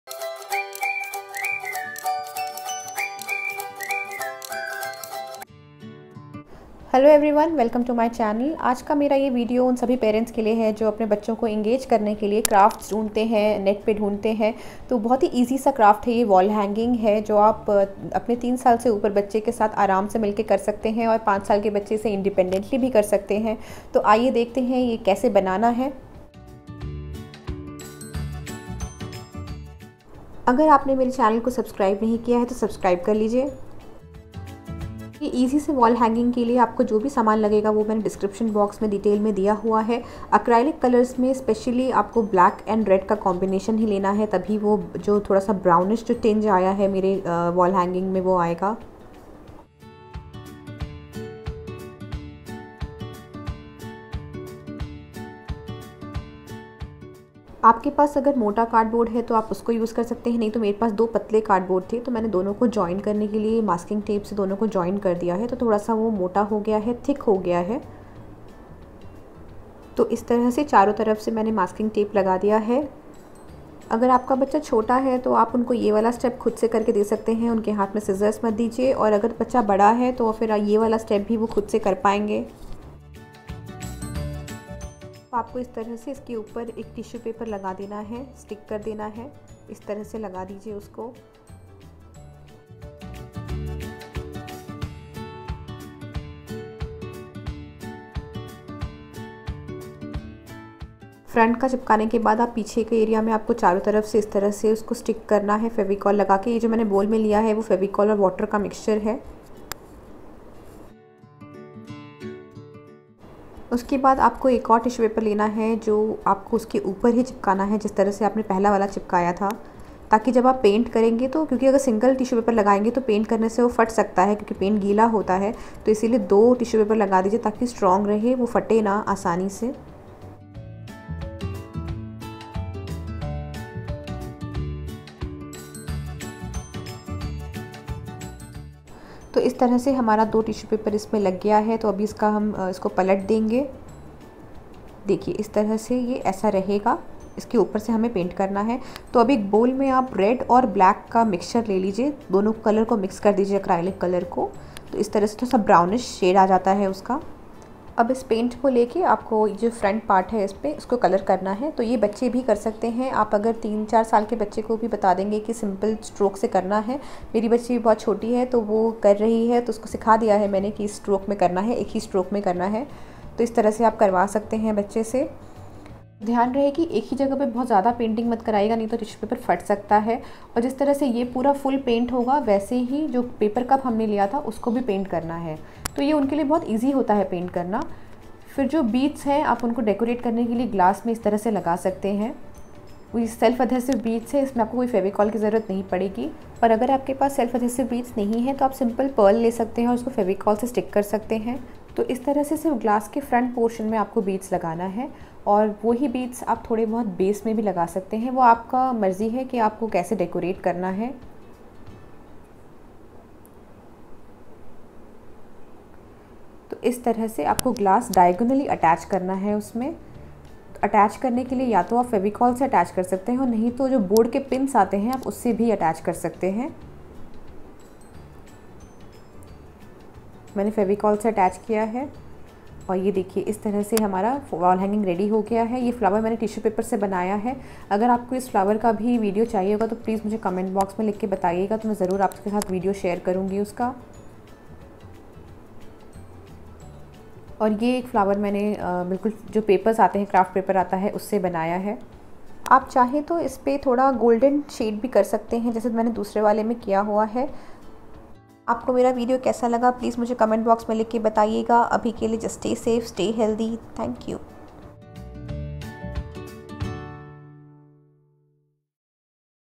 Very Easy Wall Hanging For Kids | Things To Do At Home With Kids| Easy Wall Hanging Crafts For Kids
Hello everyone
Help your child in making this easy wall hanging..aThey will definitely enjoy and it will add to your home decor

Things required
Cardboard( cut in square shape)
paper cup
acrylic colour( red and black)
tissue paper(2)
adhesive beads
masking tape
scissor
fevicol

Link to more videos from my channel

Mere aur bhi videos ke playlist ke link yahan mil jayenge

v=Iin3w2qCZOE&list=PLScG3yFINtSdRb1czxiDzUhCEadGJTHL9